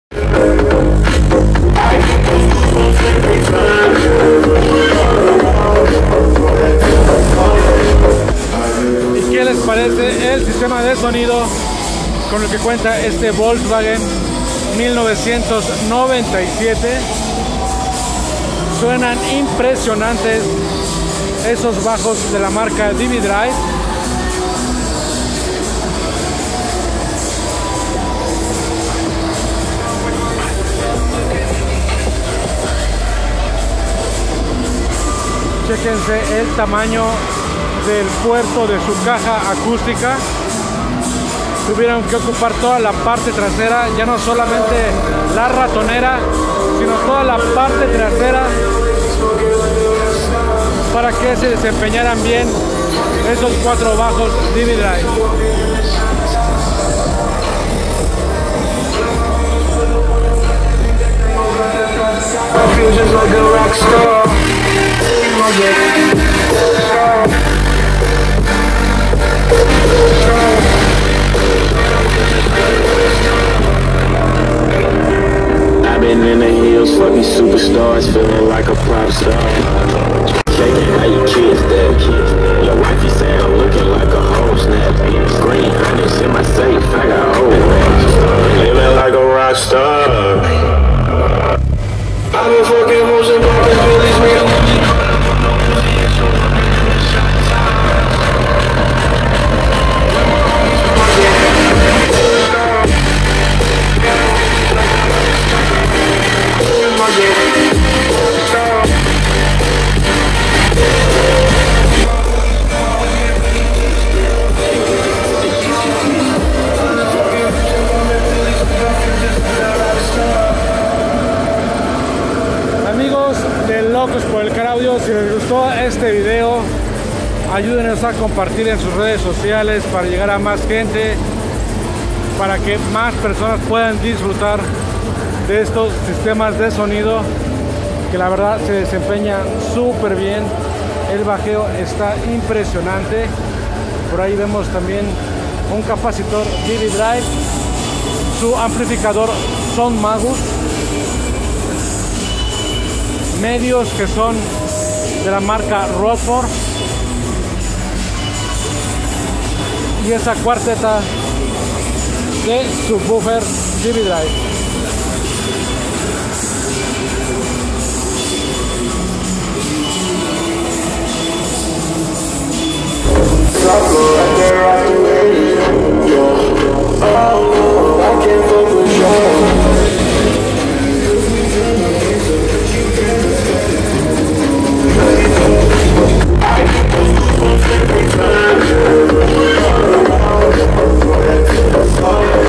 ¡Vocho 1997 con 4 SUBS DB Drive!
Gran desempeño mostró este Volkswagen en el Querétaro Wagen Show 8, realizado en el Eco Centro Expositor de Querétaro.